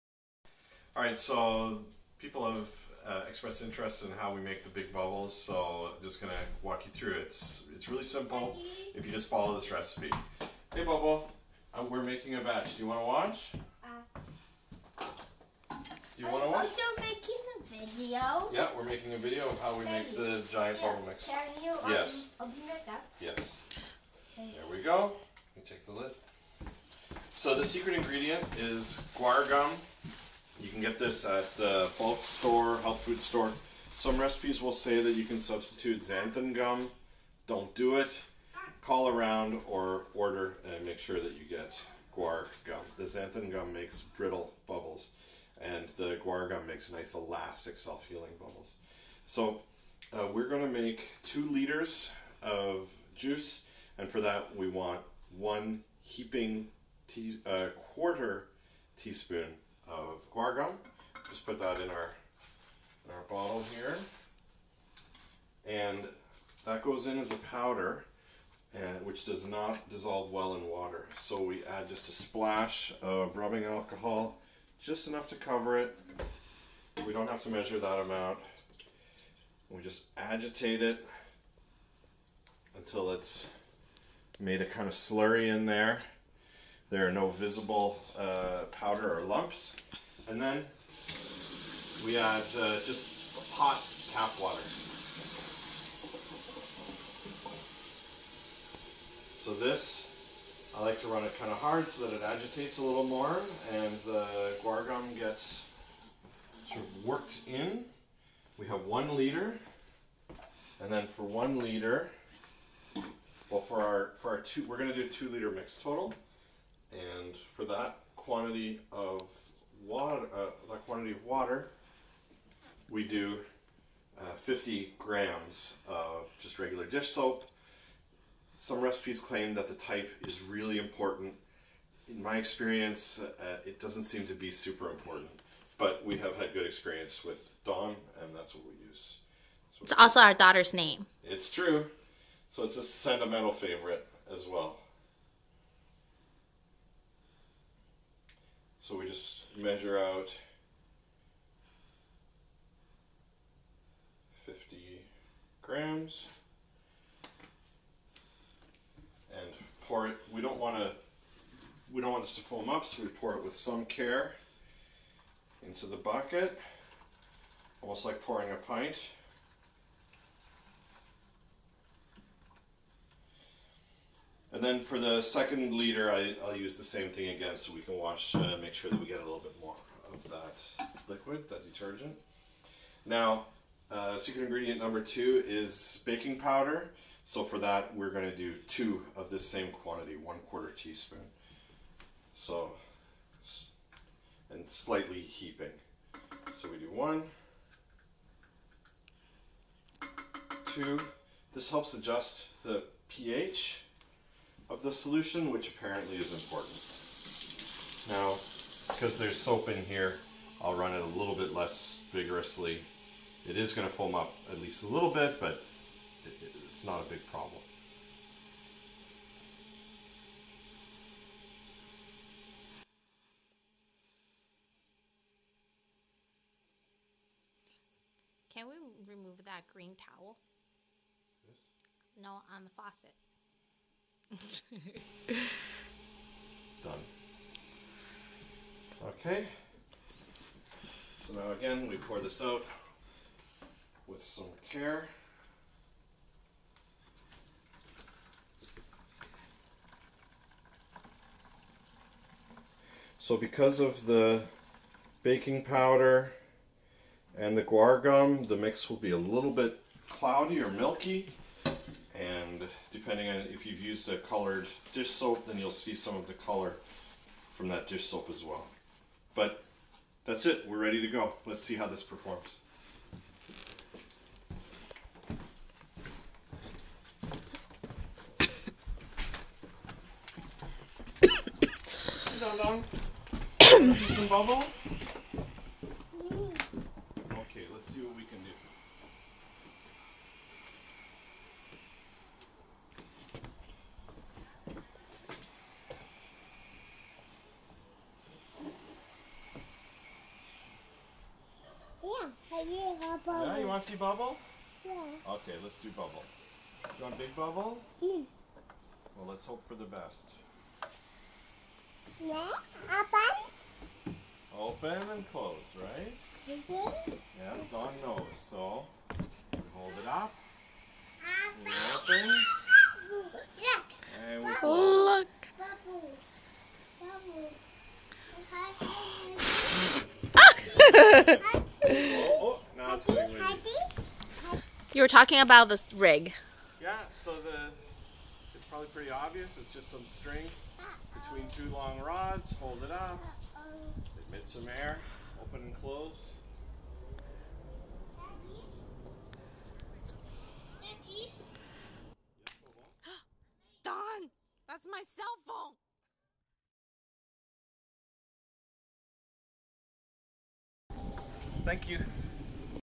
Giant Bubble Mix with Guar Gum
A quick but detailed demo of how to make the bubble juice for making large soap bubbles.